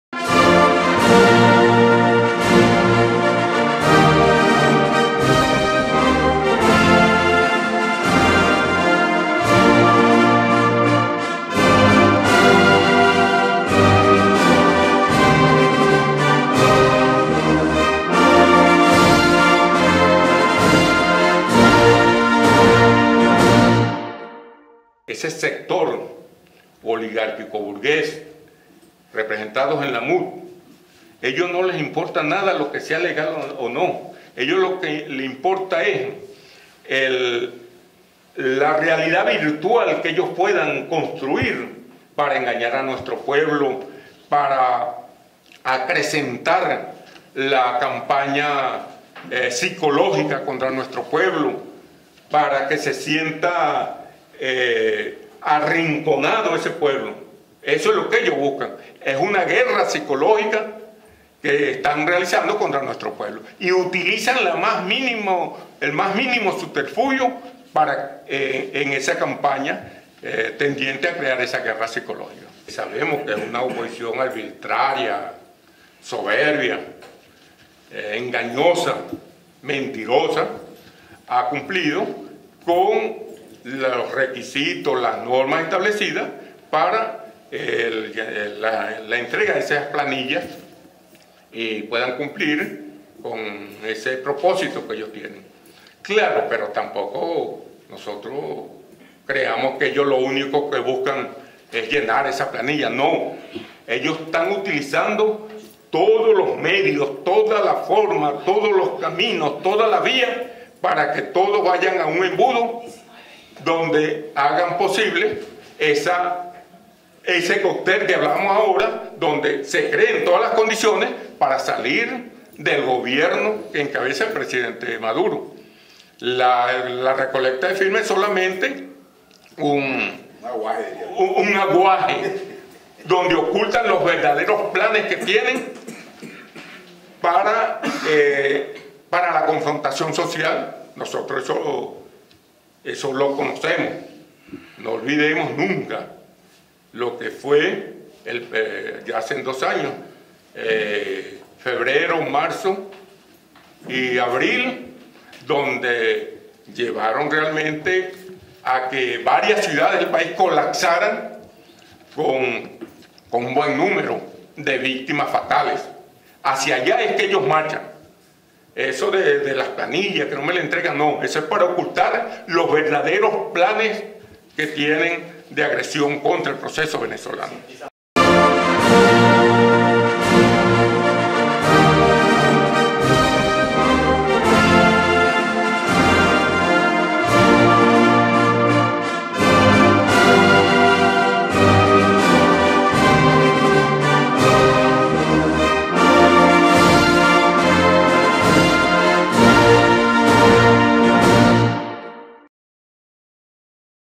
PCV: La derecha Fascista tiene una agenda de agresión al pueblo venezolano
Caracas, 18 abr. 2016, Tribuna Popular TP.- El Buró Político del Partido Comunista de Venezuela (PCV), alertó al pueblo venezolano los planes de agresión de la derecha fascista contra los logros alcanzados por el pueblo venezolano en estos 17 años y llamó a organizarse y movilizarse en defensa de los intereses del pueblo.